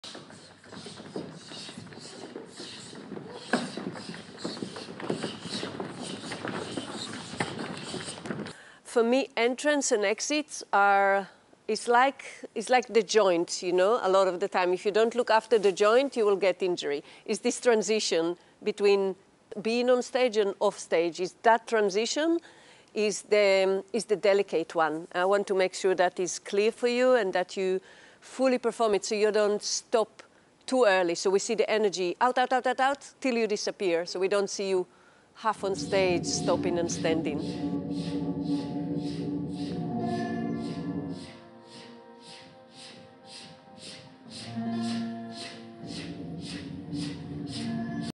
In the studio with JV2 Cohort October 24 in rehearsals for 'Tomorrow' by Jasmin Vardimon
Behind the scenes with JV2 Cohort October 24 in rehearsals for "Tomorrow" by the renowned choreographer Jasmin Vardimon.

Witness the dedication, precision, and passion as these talented dancers refine their movements, embodying the essence of Vardimon's distinctive style.

In this exclusive glimpse into the studio, you'll witness Jasmin speaking to her students about how transitions are extremely important in her work!

Join us on this journey as we delve into the rehearsal space, uncovering the artistry and commitment behind the scenes. Whether you're a dance enthusiast or simply curious about the creative process, this video offers a unique opportunity to get an insight into the rehearsal process and preparing for tour as a JV2!

🔗 If you enjoyed our content!